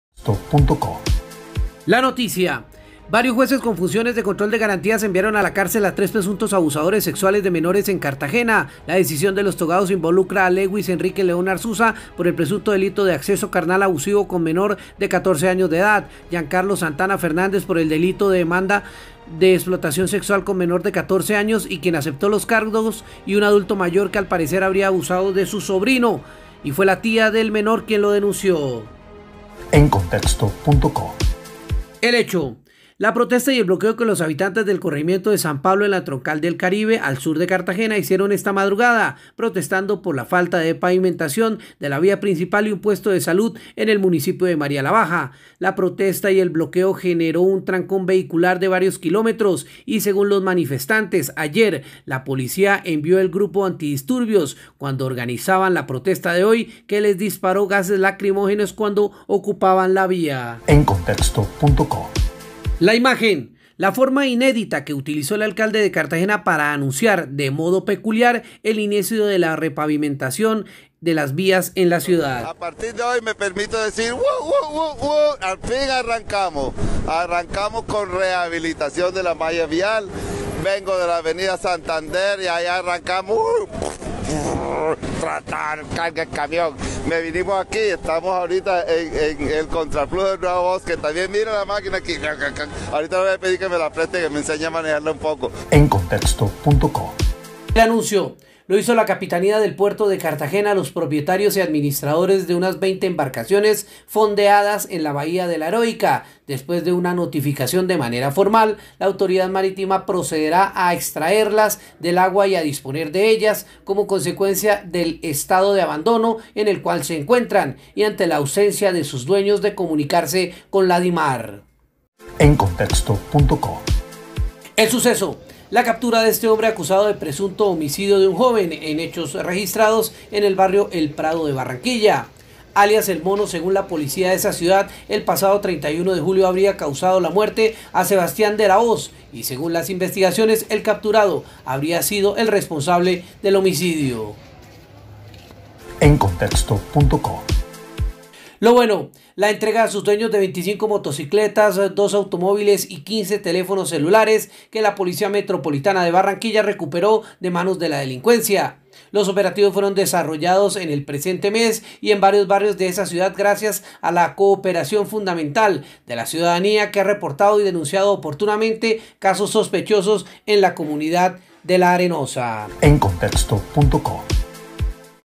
Jueces de Cartagena envía a la cárcel a tres presuntos abusadores sexuales.
El resumen de las noticias se ve mejor en la actualización de Encontexto.co